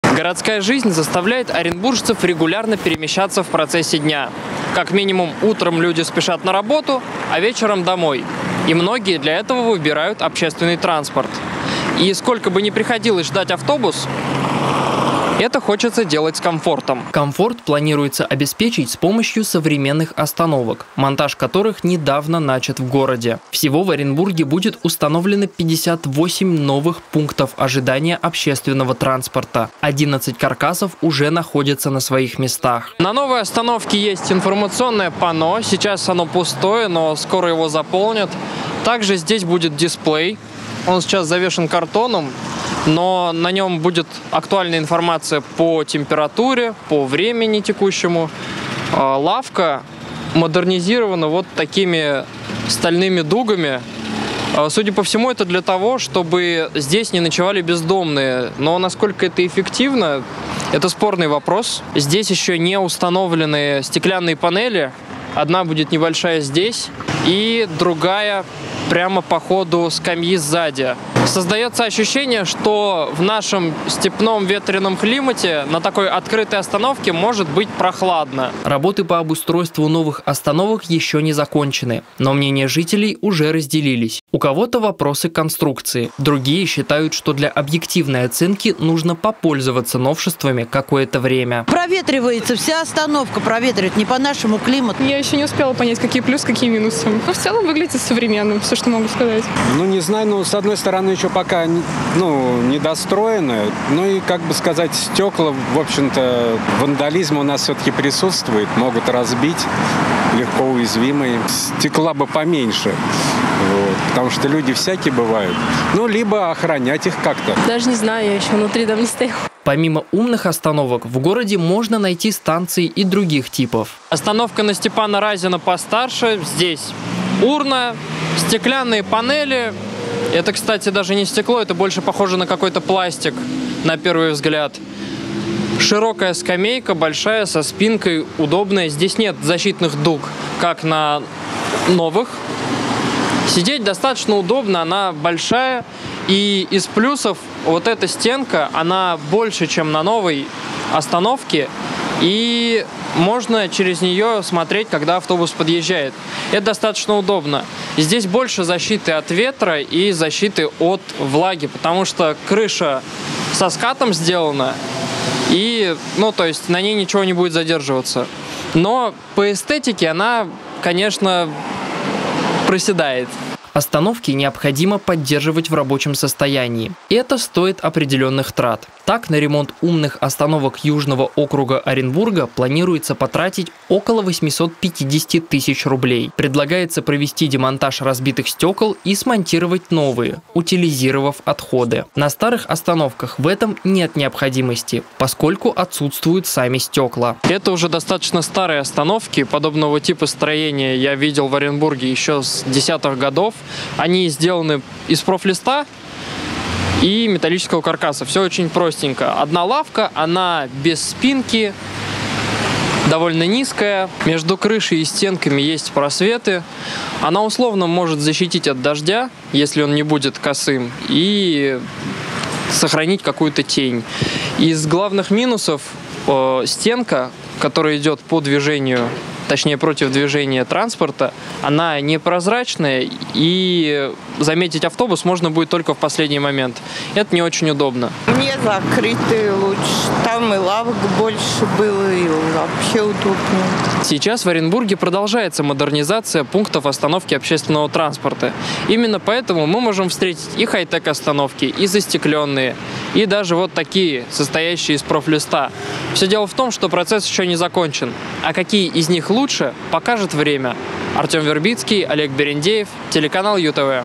В Оренбурге на проспекте Победы начали устанавливать новые остановки
К монтажу готовы 25 новых павильонов. Сколько всего остановок обновят в городе и чем они отличаются от старых, расскажет наш корреспондент.